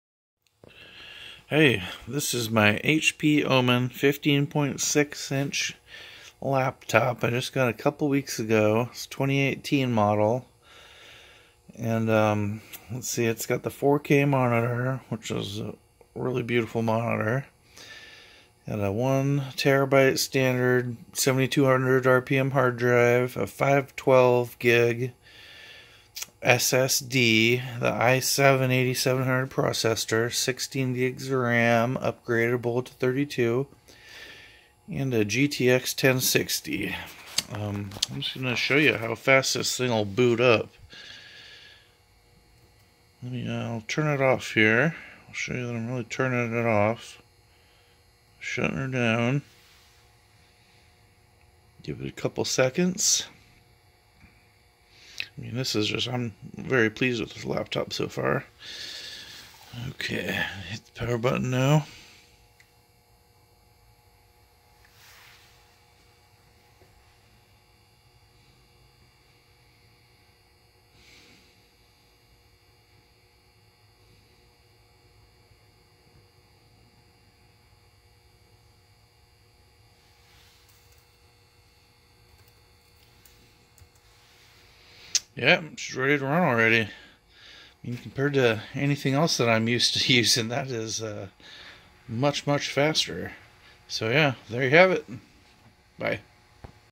Hp Omen 2018 15.6" 4k model boot up speed. Model 15-dc0052nr 2018
Hp Omen  15-dc0052nr boot up speed.

Product specifications

Operating system
Windows 10 Home 64

Processor
Intel® Core™ i7-8750H (2.2 GHz base frequency, up to 4.1 GHz with Intel® Turbo Boost Technology, 9 MB cache, 6 cores) [20,21]

Graphics
Discrete: NVIDIA® GeForce® GTX 1060 (6 GB GDDR5 dedicated)

Display
15.6" diagonal 4K IPS anti-glare micro-edge WLED-backlit (3840 x 2160) [15]

Memory
16 GB DDR4-2666 SDRAM (1 x 16 GB)

Storage
1 TB 7200 rpm SATA [16]

Secondary storage
512 GB PCIe® NVMe™ M.2 SSD [17,18]

Network interface
Integrated 10/100/1000 GbE LAN

Wireless technology
Intel® Wireless-AC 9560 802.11b/g/n/ac (2x2) Wi-Fi® and Bluetooth® 5 Combo [13,14]

Power supply
200 W AC power adapter

Battery
4-cell, 70 Wh Li-ion

Battery life
Up to 6 hours

Video Playback Battery Life
Up to 4 hours and 30 minutes

External I/O Ports
1 headphone/microphone combo; 1 microphone-in; 1 Mini DisplayPort™; 1 RJ-45; 1 Thunderbolt™ 3 (Data Transfer up to 40 Gb/s, DP1.2, HP Sleep and Charge); 3 USB 3.1 Gen 1 (1 HP Sleep and Charge); [8,9,10,11]

Expansion slots
1 multi-format SD media card reader

Energy efficiency
ENERGY STAR® certified; EPEAT® Silver registered

Webcam
HP Wide Vision HD Camera with integrated dual array digital microphone [7]

Audio
Bang & Olufsen, dual speakers, HP Audio Boost, DTS Headphone:X™ support [11]

Special features
NVIDIA G-SYNC™ technology

Color
Shadow black

Dimensions (W X D X H)
14.17 x 10.35 x 1.02 in

Weight
5.56 lb

Warranty
90 day phone support (from date of purchase); complimentary chat support within warranty period

Software included
McAfee LiveSafe™

Netflix [5][22]

Security management
Kensington NanoSaver™ Security Lock Support